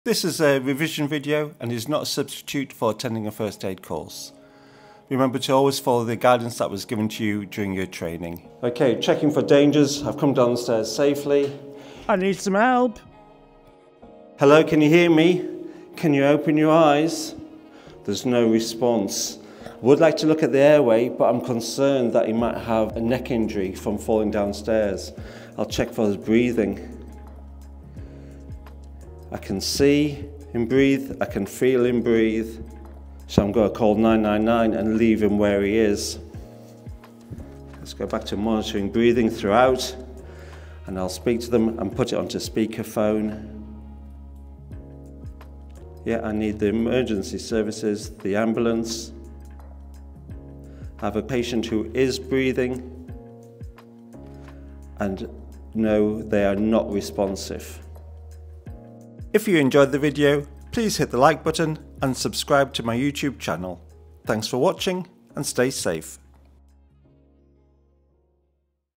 First Aid skills. Spinal Injury. How to carry out a Primary Survey (aka DRABC).
The Primary Survey (aka DRABC) is the first sequence of actions undertaken by a first aider who comes across an emergency. The rationale is to ensure safety for everyone, particularly yourself, and then assess the severity of the situation. The outcome of the assessment will then determine the next steps to be taken.
In this case our patient has apparently fallen down stairs, is unconscious and  we must suspect a spinal injury. therefor extreme caution needs to be taken.
This video will walk you through the modified steps of performing a Primary Survey to minimise the risk of making the injury worse.
D-Dangers. Check for dangers that threaten your safety. You must put your own safety first, then the safety of others, then the safety of the patient. You cannot carry out first aid in a situation where you yourself, or others may also become casualties.
R-Response. Check for a response from the patient. This is a quick test and identifies if the person's brain is working OK! If the person can't respond well, their brain function is lowered. If the brain function is so low that they are unconscious then the patients airway can easily get blocked.
A-Airway. Moving the patients head could make the injury worse. As long the patient does not have an airway problem, then I'll leave the head alone and assess the breathing.
B-Assess the Breathing. If the person is not breathing normally then you must start resuscitation, even if you suspect a neck injury. Resuscitation is covered in a separate video.
C-Check the Circulation. You can look for blood loss. You can also feel the forehead. If the person is bleeding, stop the bleeding. If the skin is cold and clammy, report this to the 999 call taker. Stopping blood loss and dealing with 'shock' are covered in other videos.
That's the Primary Survey complete. As help has not arrived I need to make a 999 call and keep monitoring the patient.